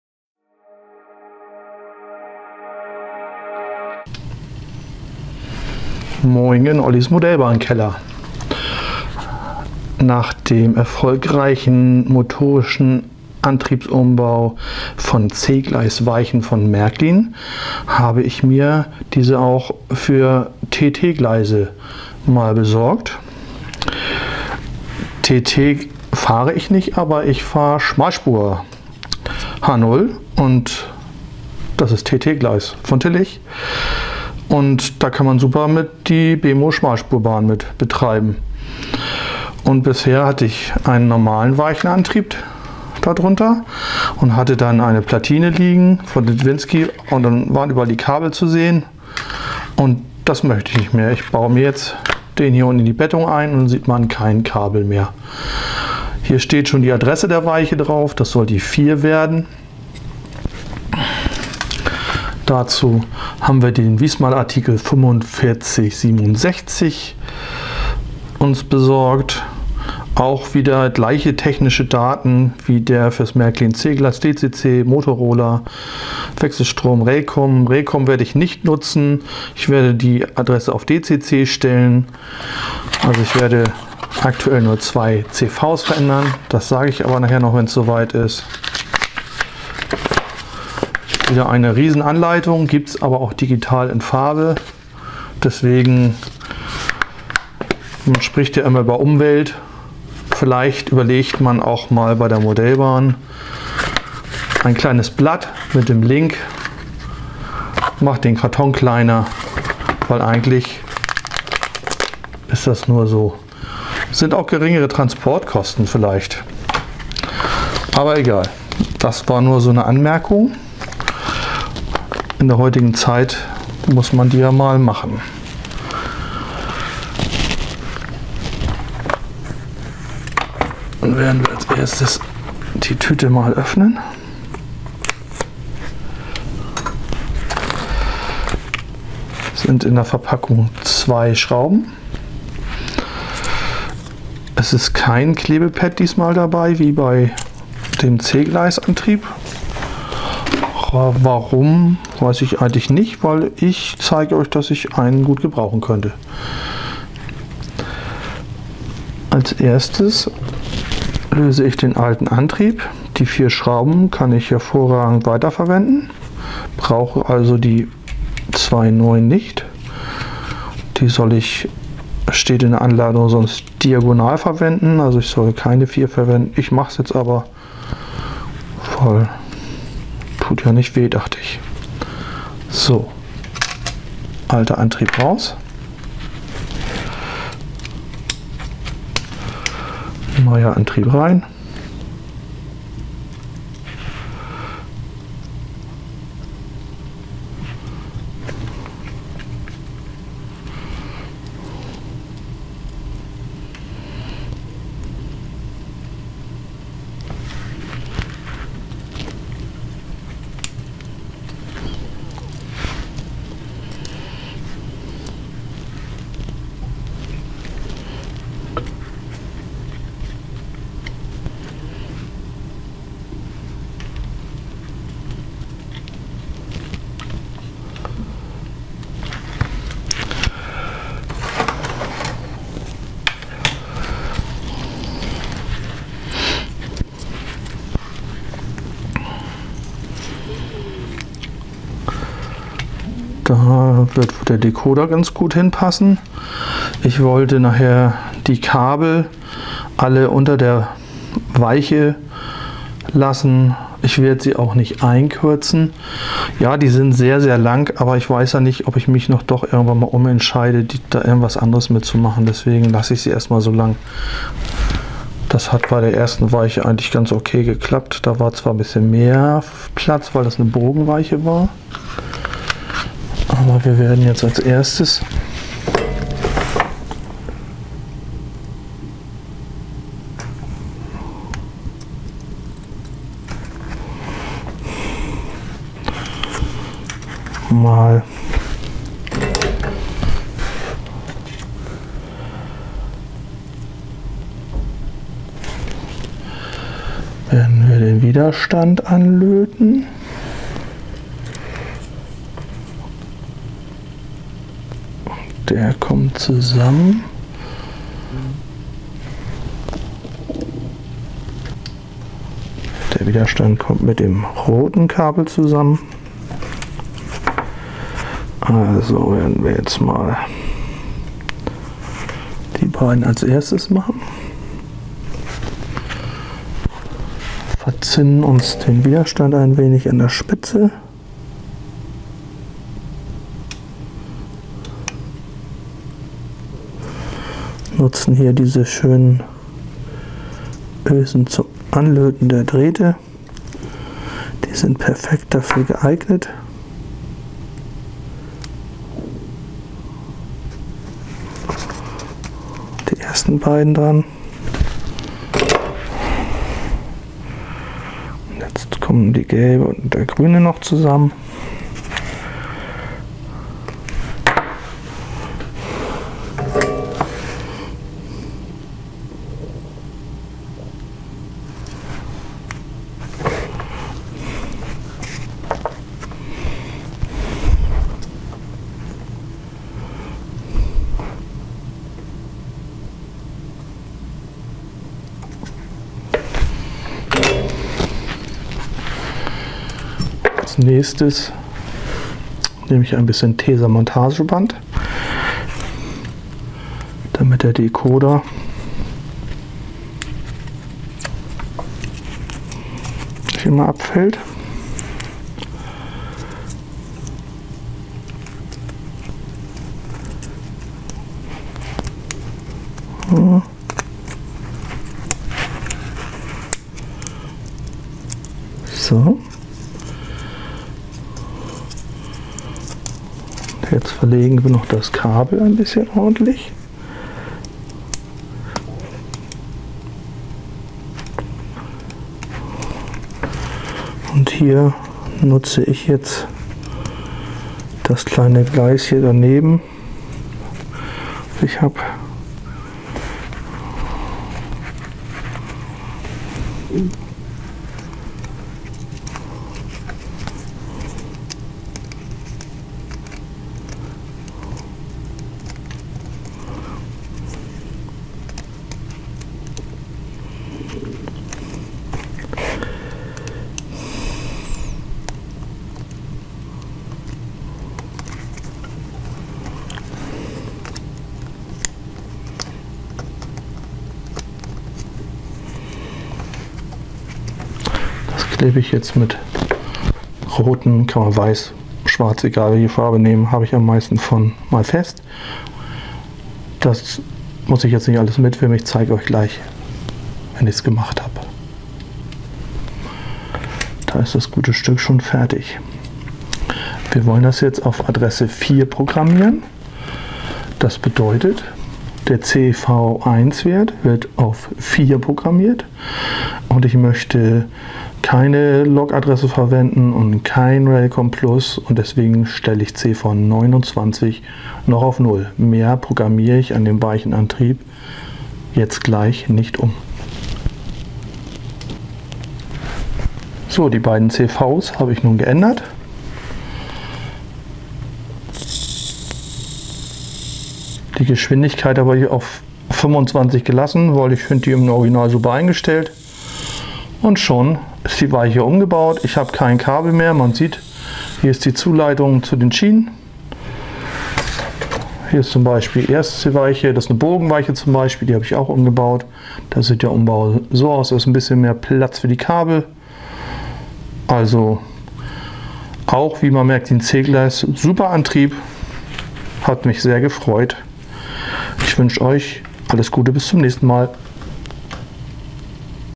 Viessmann 4567 Weichenantrieb TT Vorstellung
Hier möchte ich auch den neuen Viessmann Weichenantrieb vorstellen und den Einbau zeigen